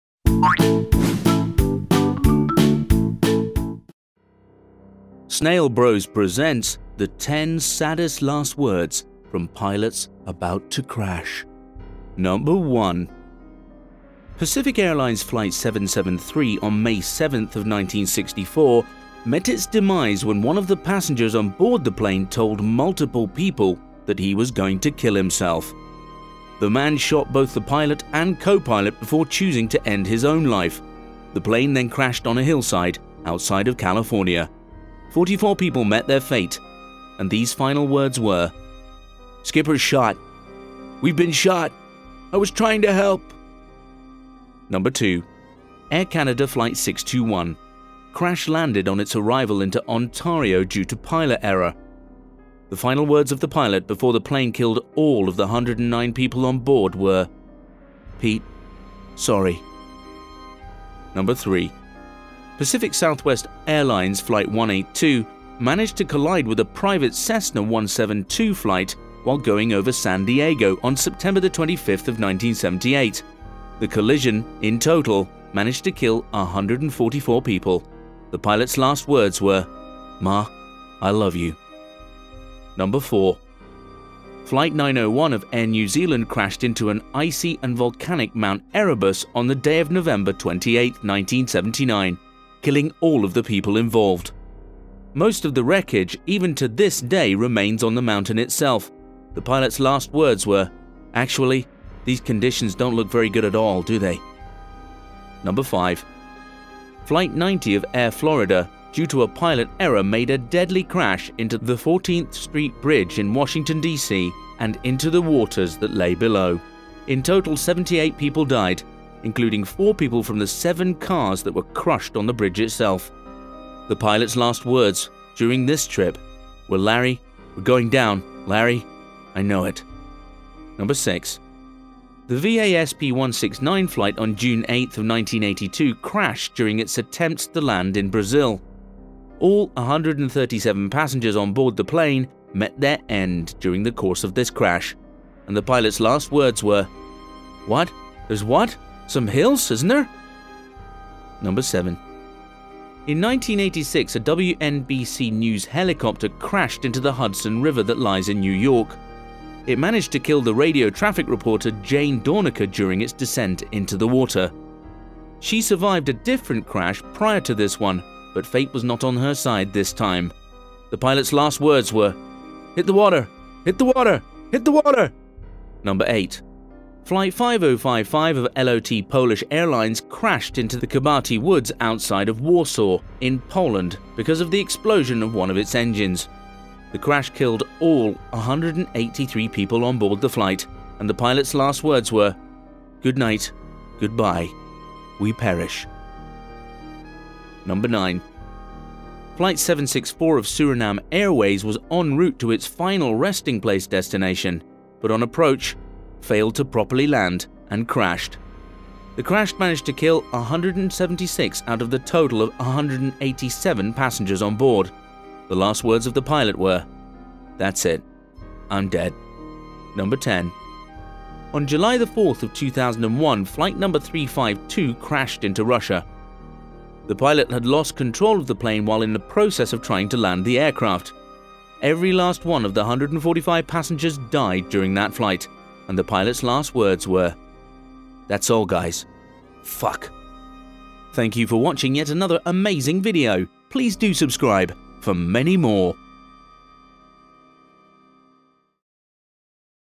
10 Saddest Last Words From Pilots About to Crash ! (PART 2)
10 Saddest Last Words From Pilots About to Crash

Number One: Pacific Airlines flight 773, on May 7th of 1964, met its demise when one of the passengers on board the plane told multiple people that he was going to kill himself. The man shot both the pilot and copilot before choosing to end his own life. The plane then crash on a hillside outside of California.  44 people met their fate, and these final words were: "Skipper's shot!  We've been shot.  I was trying to help"

Number Two: Air Canada, flight 621, crash landed upon its arrival into Ontario due to a pilot error. The final words of the pilot, before the plane killed all of the 109 people on board were: "Pete, sorry."

Number Three: Pacific Southwest Airlines, flight 182, managed to collide with a private Cessna 172 flight while going over San Diego on September 25th of 1978. The collision, in total, managed to kill 144 people.  The pilot's last words were: "Ma, I love you"

Number Four: Flight 901 of Air New Zealand crashed into the icy and volcanic Mount Erebus on the day of November 28th, 1979, killing all of the people involved.
Most of the wreckage, even to this day, remains on the mountain itself.  The pilot's last words were: "Actually, these conditions don't look very good at all, do they?"

Number Five: Flight 90 of Air Florida, due to a pilot error, made a deadly crash into the 14th street bridge in Washington, D.C., and into the waters that lay below. In total, 78 people died, including 4 people from the seven cars that were crushed on the bridge itself.  The pilot's last words during this trip were "Larry, we're going down, Larry... I know it!"

Number Six: The VASP 169 flight on June 8th of 1982 crashed during its attempts to land in Brazil. All 137 passengers on board the plane met their end during the course of this crash, and the pilot's last words were "What?  There's what?  Some hills, isn't there?"

Number Seven: In 1986 a WNBC news helicopter crashed into the Hudson River that lies in New York.  It managed to kill the radio traffic reporter Jane Dornacker during its descent into the water. She survived a different crash prior to this one, but fate was not on her side this time.  The pilot's last words were "hit the water...hit the water...hit the water"

Number Eight: Flight 5055 of LOT Polish airlines crashed into the Kabaty Woods outside of Warsaw in Poland because of the explosion of one of its engines. The crash killed all 183 people on board the flight, and the pilot's last words were: "Goodnight, goodbye, we perish"

Number Nine: Flight 764 of Surinam Airways was en route to it's final resting place destination, but on approach, failed to properly land and crashed. The crash managed to kill 176 out of the total 187 passengers on board.  The last words of the pilot were: "That's it. I'm dead."

Number Ten: On July 4th of 2001, flight number 352 crashed into Russia.  The Pilot had lost control of the plane while in the process of trying to land the air craft. Every last one of the 145 passengers died during that flight, and the pilot's last words were: "That's all guys! Fuck!"